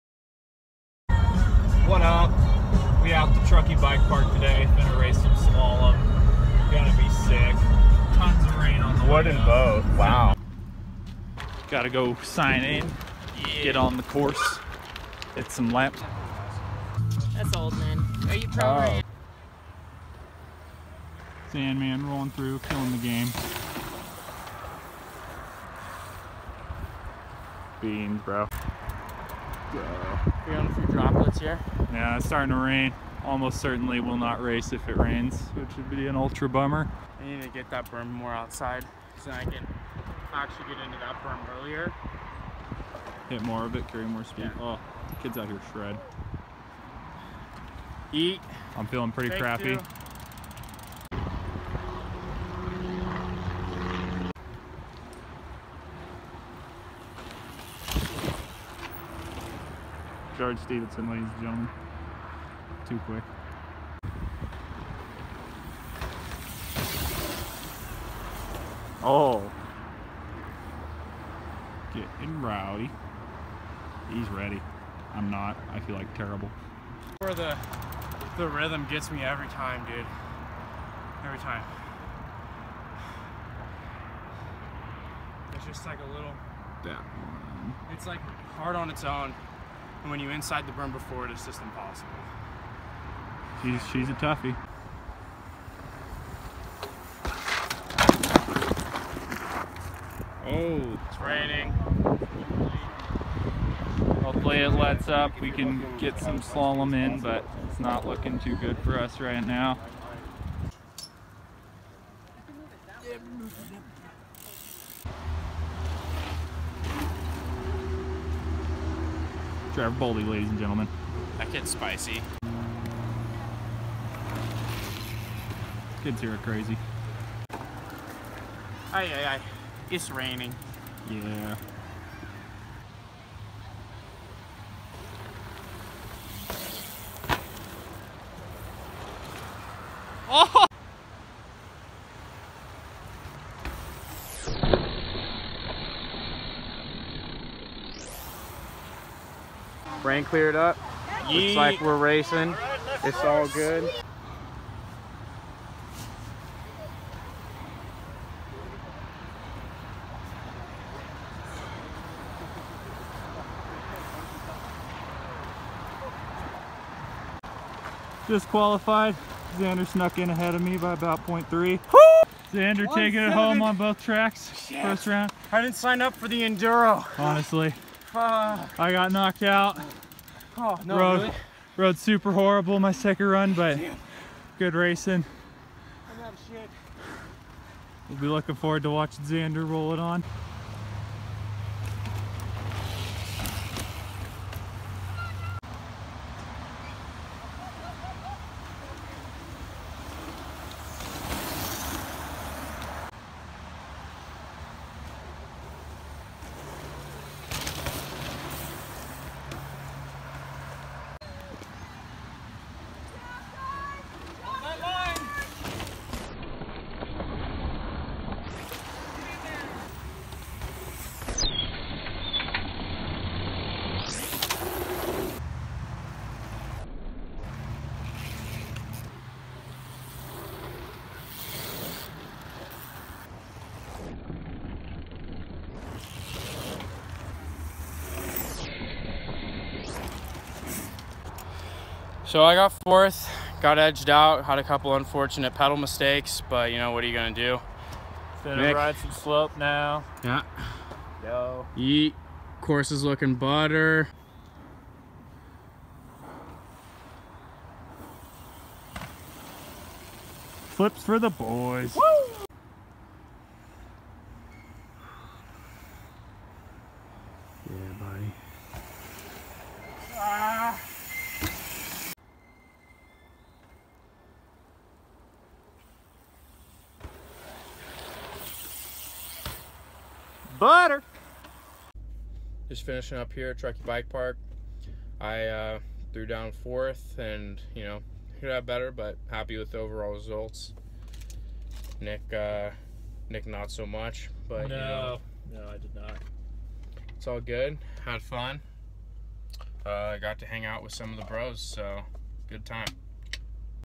MTB DUAL SLALOM RACING IN THE RAIN
Dual Slalom racing at Truckee Bike Park!

Featuring: Nick Azevedo, Xander Rosenthal